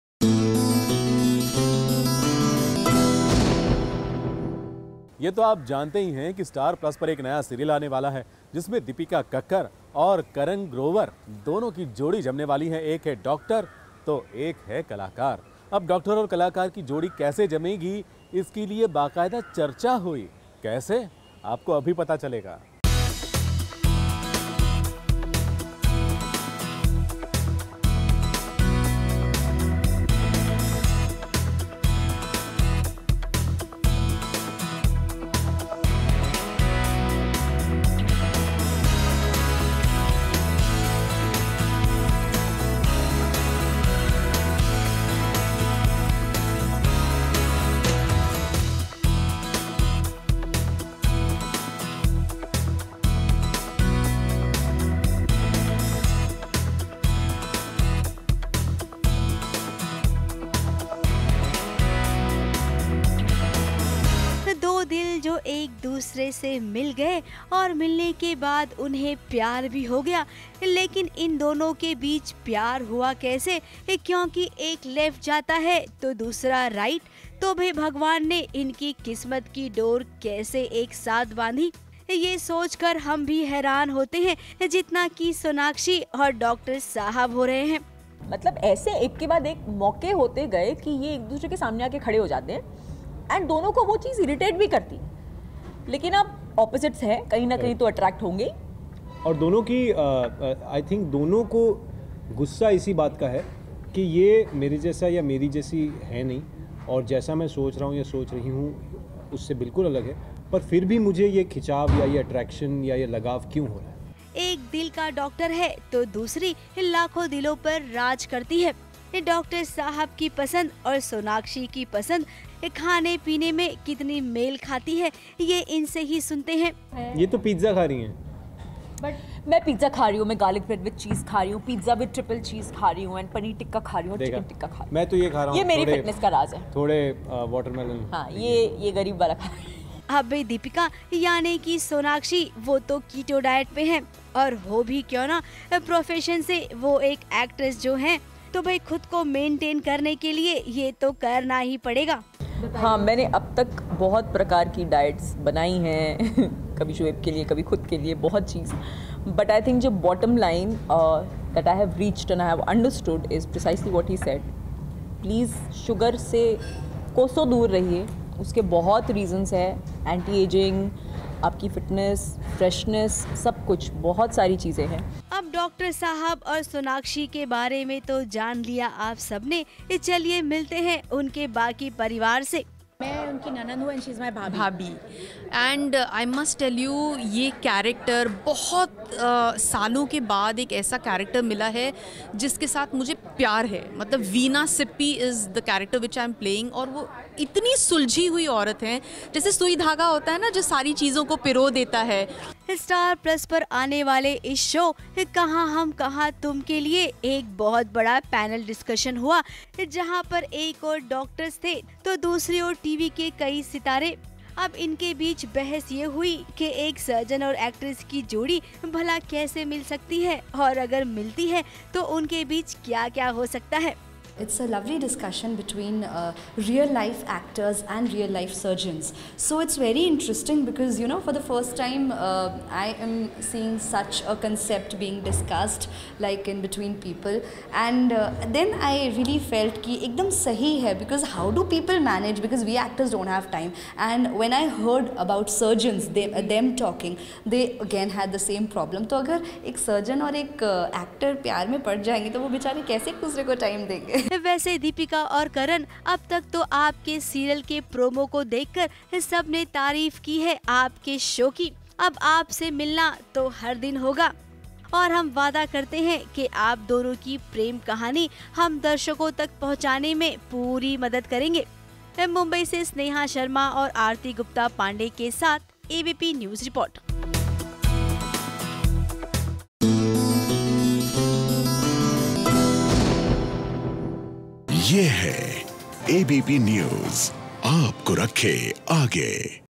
Dipika Kakar, Karan V Grover's New Show 'Kahan Hum Kahan Tum' Gets Launched
Dipika Kakkar and Karan V Grover interacted with doctors and surgeons along with well-known television actors who all discussed the complexities of the two different industries.